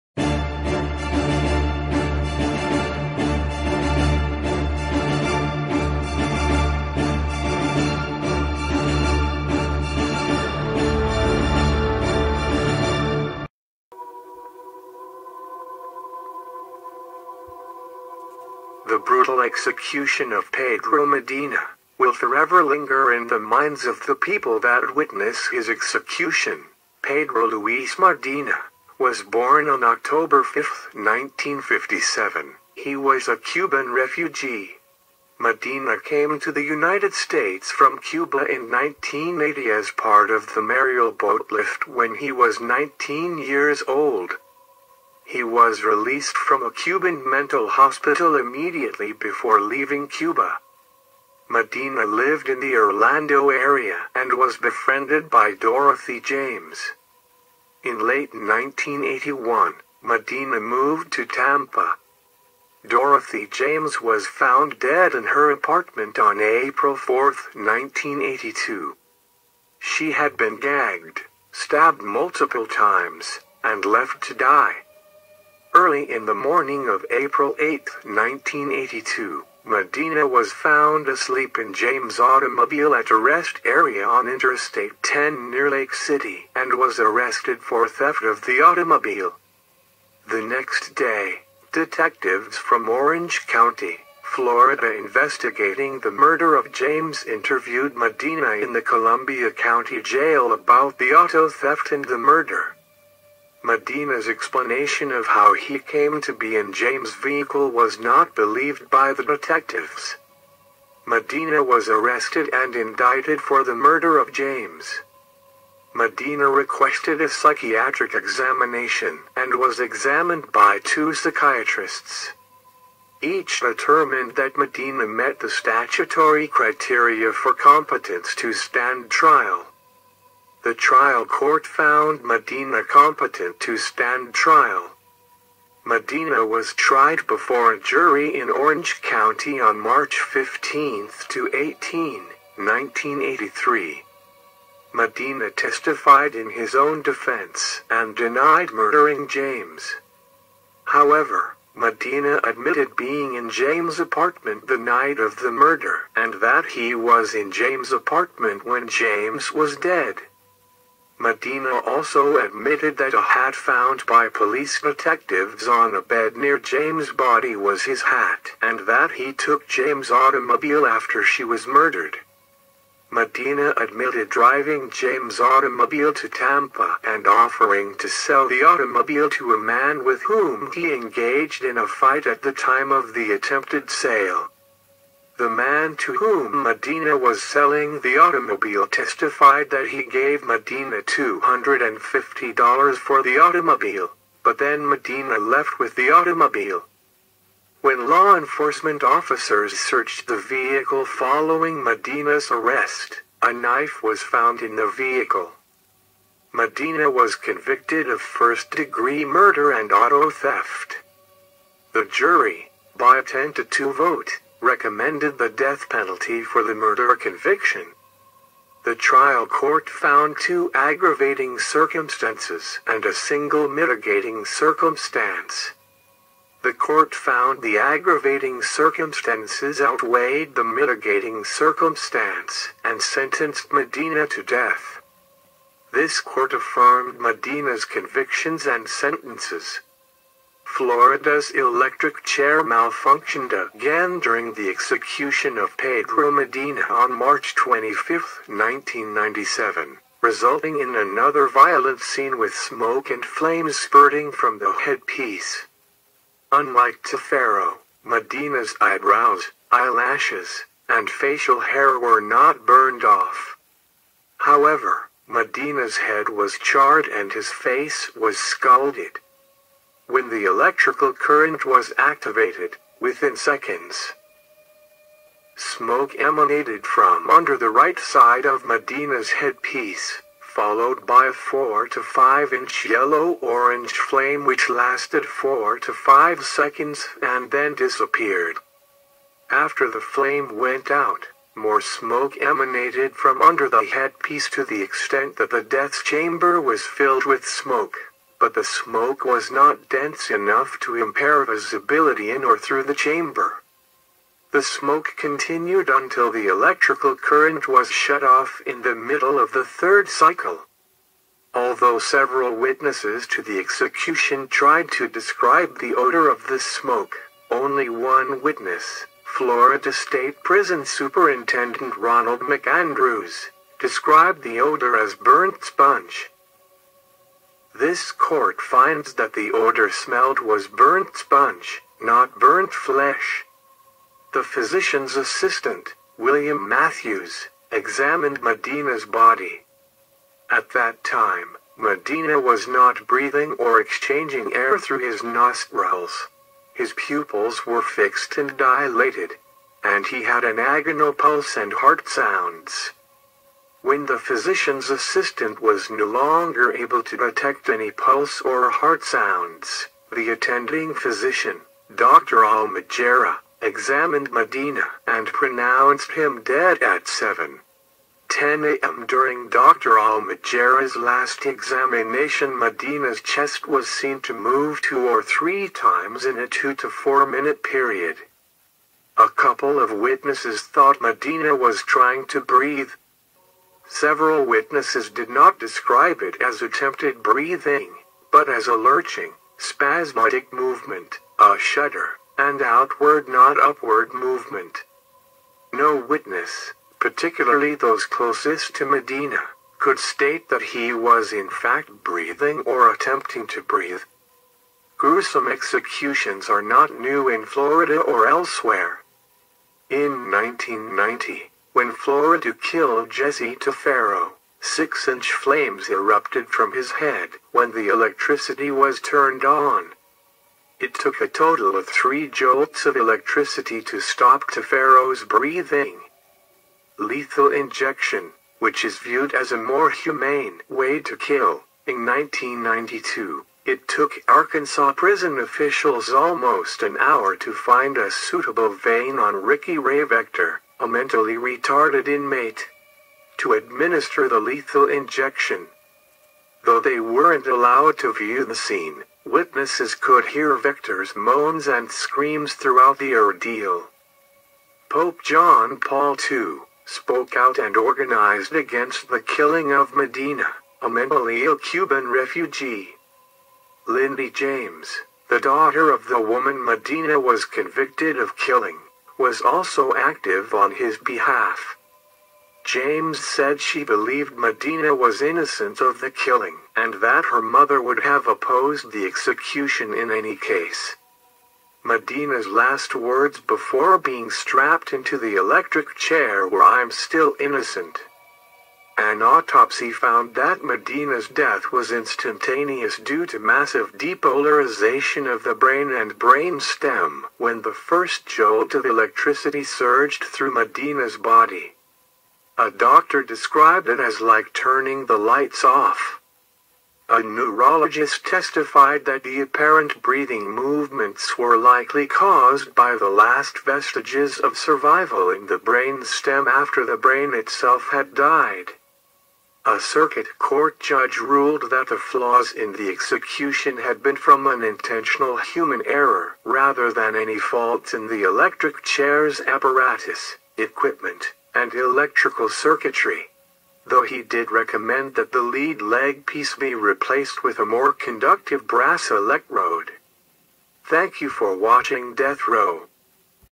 THE BRUTAL EXECUTION OF - Pedro Medina
Medina was among nearly 125,000 Cubans who were sent to the United States during the 1980 Mariel boatlift. He eventually lived with his half-sister in Orlando. His victim, Dorothy James, an elementary school gym teacher, lived in an apartment next door. James befriended Medina.

Dorothy James was found dead in her apartment on April 4, 1982. She had been gagged, stabbed multiple times, and left to die. Early in the morning of April 8, 1982, Medina was found asleep in James' automobile at a rest area on Interstate 10 near Lake City and was arrested for theft of the automobile. The next day, detectives from Orange County, Florida investigating the murder of James interviewed Medina in the Columbia County Jail about the auto theft and the murder. Medina's explanation of how he came to be in James' vehicle was not believed by the detectives. Medina was arrested and indicted for the murder of James. Medina requested a psychiatric examination and was examined by two psychiatrists. Each determined that Medina met the statutory criteria for competence to stand trial. The trial court found Medina competent to stand trial.

Medina was tried before a jury in Orange County on March 15–18, 1983. Medina testified in his own defense and denied murdering James. However, Medina admitted being in James' apartment the night of the murder and that he was in James' apartment when James was dead. Medina also admitted that a hat found by police detectives on a bed near James' body was his hat and that he took James' automobile after she was murdered. Medina admitted driving James' automobile to Tampa and offering to sell the automobile to a man with whom he engaged in a fight at the time of the attempted sale. The man to whom Medina was selling the automobile testified that he gave Medina $250 for the automobile, but then Medina left with the automobile. When law enforcement officers searched the vehicle following Medina's arrest, a knife was found in the vehicle.

Medina was convicted of first-degree murder and auto theft. The jury, by a ten-to-two vote, recommended the death penalty for the murder conviction. The trial court found two aggravating circumstances  and a single mitigating circumstance. The court found the aggravating circumstances outweighed the mitigating circumstance and sentenced Medina to death. This Court affirmed Medina's convictions and sentences. (Medina v. State, 466 So.2d 1046 (Fla. 1985)).

He was sent to death row at Florida State Prison near the town of Starke in 1982. Medina's last words before being executed on March 25, 1997, were, "I am still innocent."[1] During the administration of electric current, the electric chair known as "Old Sparky" malfunctioned, causing flames to shoot out of Medina's head.[2]